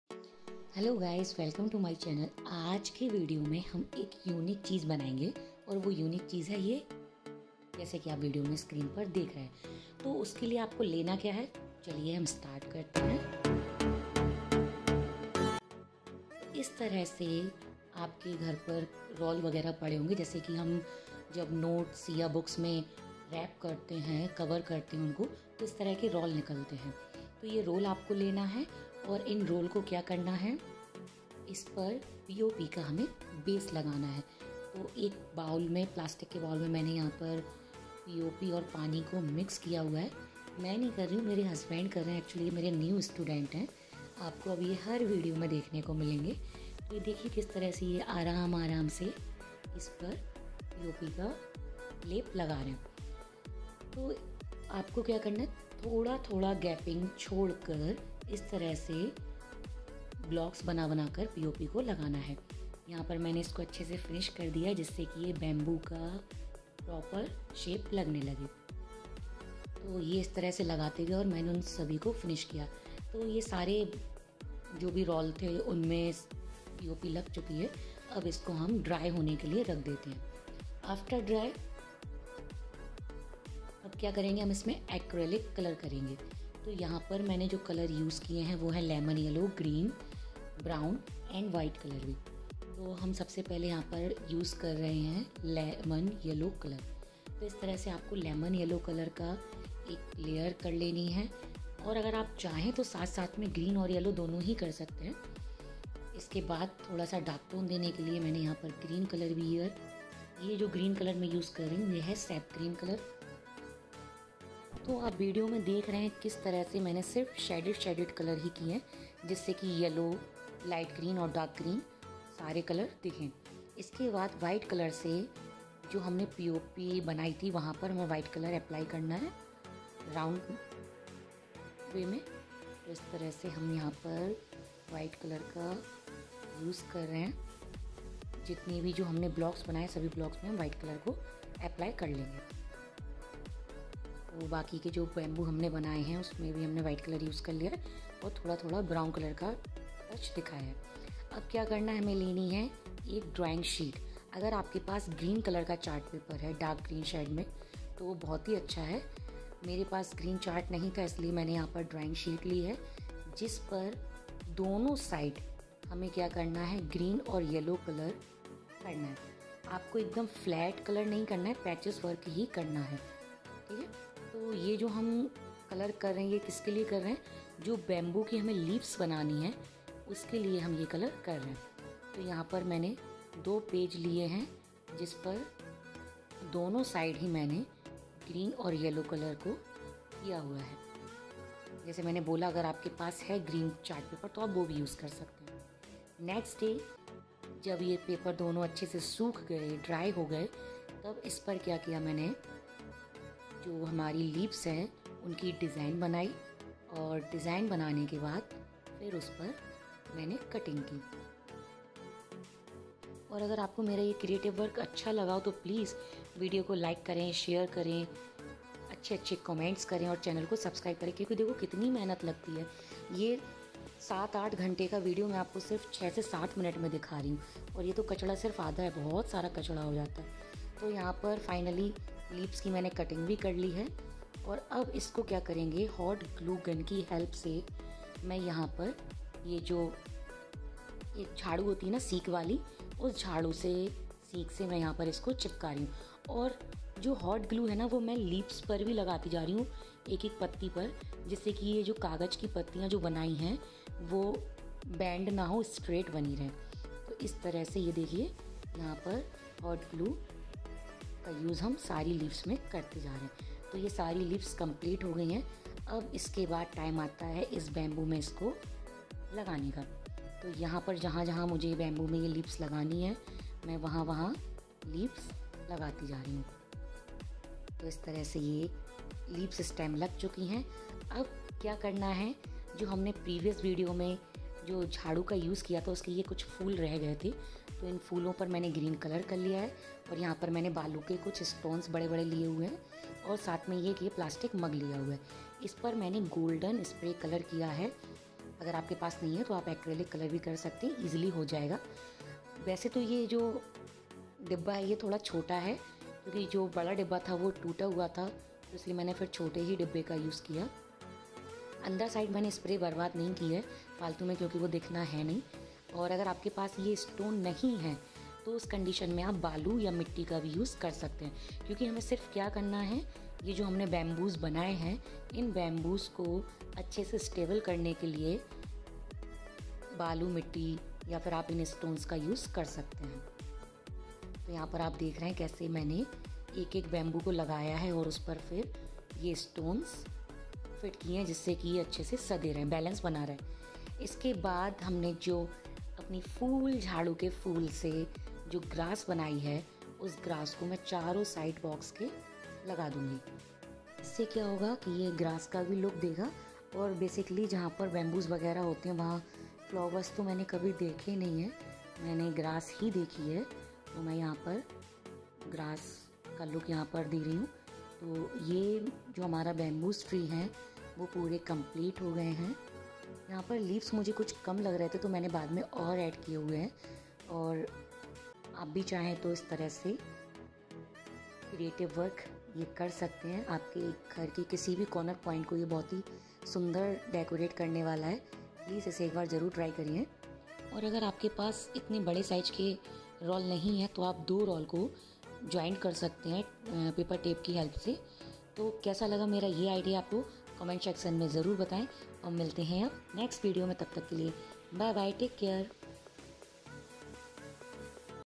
BambooTree At Home | बेकार सामान से बनाए Bamboo Home Decor Ideas| Decoration Waste Material Ideas
अब अपने घर और कमरे को सजाएं अपने हाथो से बनाए हुए सामान से।
BambooTree At Home | बेकार सामान से बनाए Bamboo tree| Decor Your Home| Waste Material Ideas|
Bamboo Home Decoration Ideas

Hello doston
मैं आपके लिए एक ऐसा ही video लेके आई हूं जिससे आप अपने घर या कमरे को सजा सकते है।

तो video को last तक देखे enjoy करें,
video को like और share भी करे,